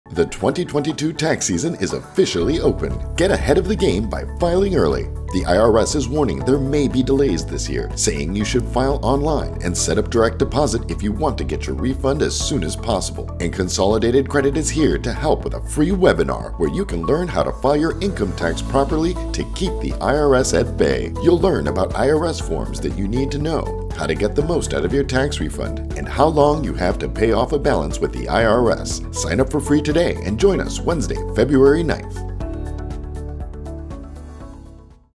Filing Income Taxes - Sneak Peek of New Free Webinar
Tax season 2022 is officially open! You can now file your 2021 annual income tax returns anytime between now and April 18, 2022. But the IRS is encouraging people to file early this year or refunds could be delayed! Consolidated Credit is here to help with a free webinar on February 9. Watch this video to get a sneak peek of what we’ll cover!

Are you confused about filing this year? Ask your question or share tips in the comments!